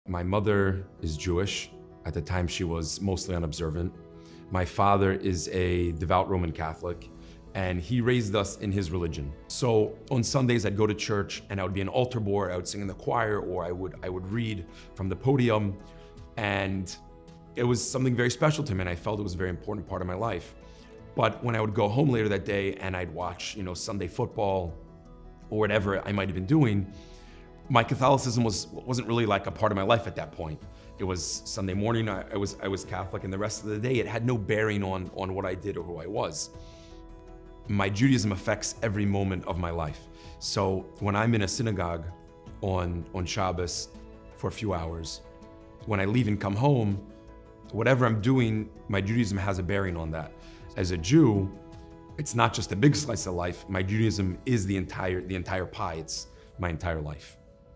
How I Became an Orthodox Jew
Rabbi Matisyahu Devlin was raised as a Catholic and now practices Orthodox Judaism. We asked him to tell us his story.

ABOUT JLI